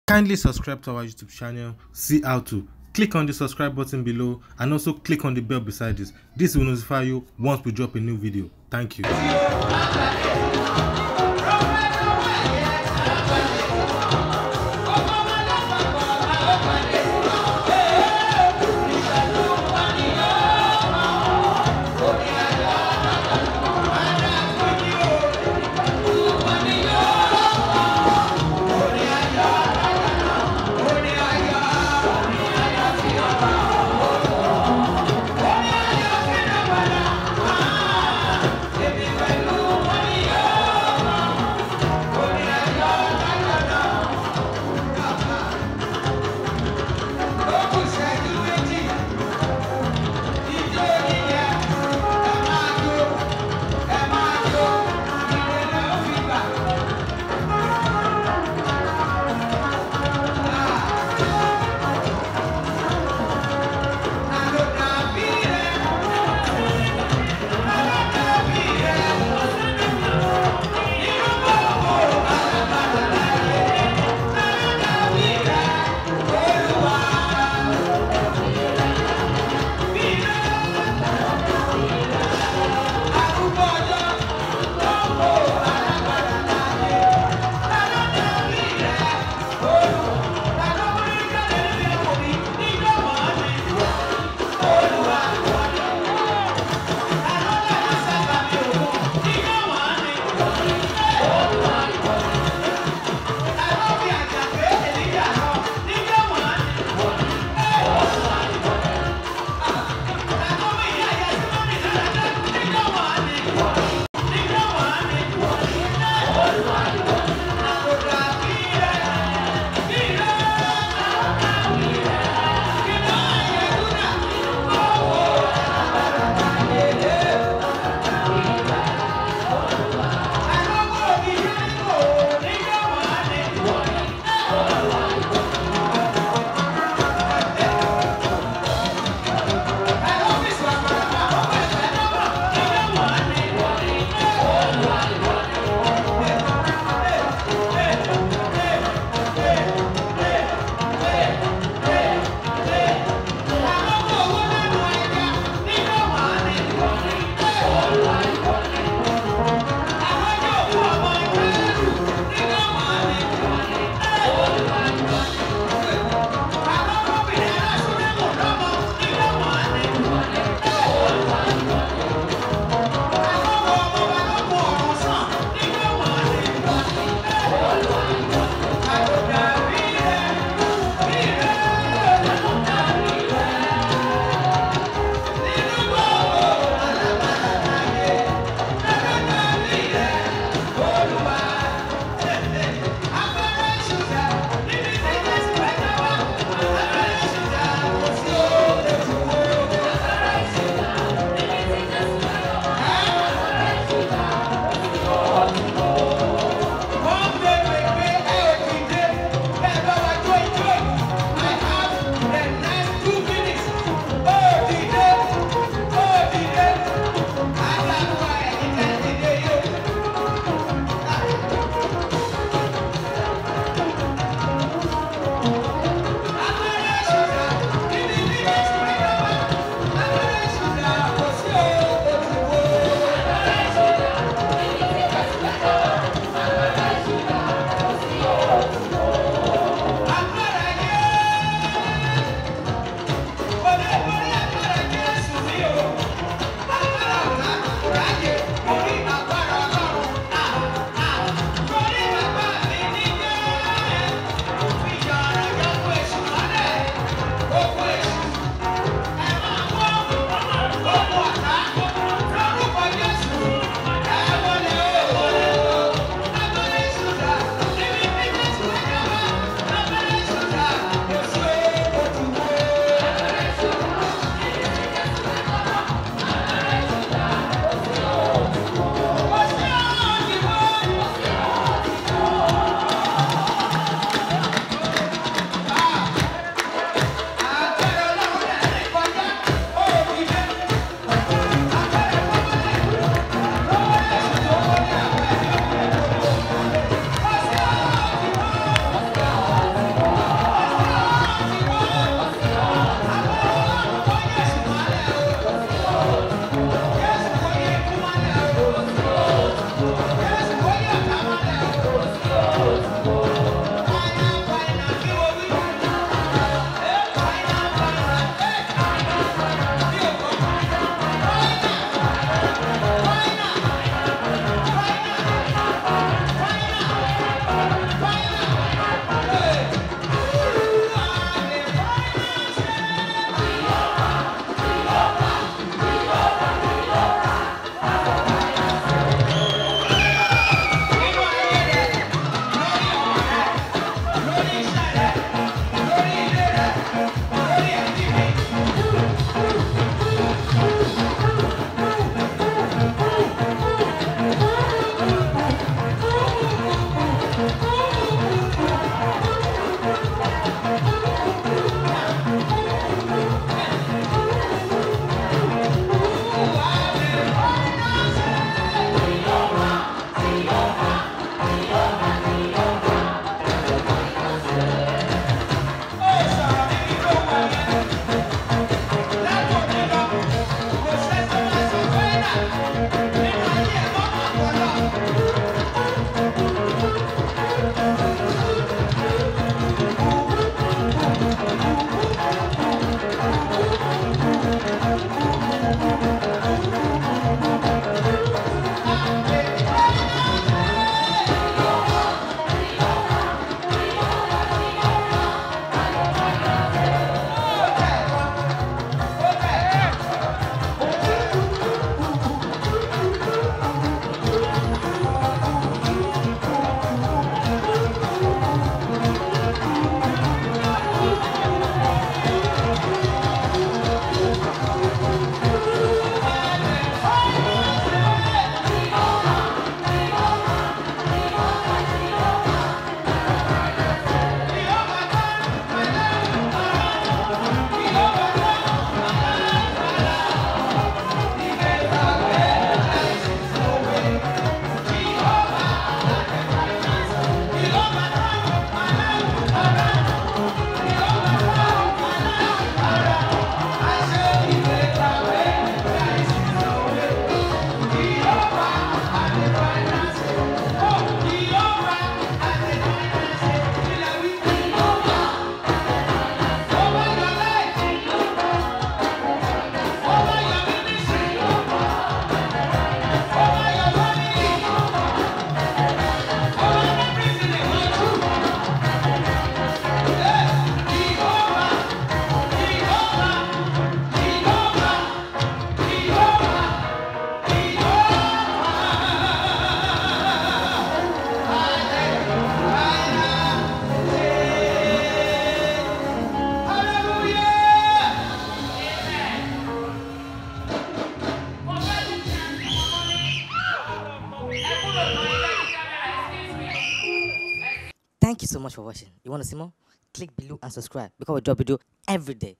Baba Sala's Daughter &friends shows off their Shaku Shaku dance steps at her father tribute concert
See Baba Sala's Daughter and friends shows off their Shaku Shaku dance steps at her father mega tribute concert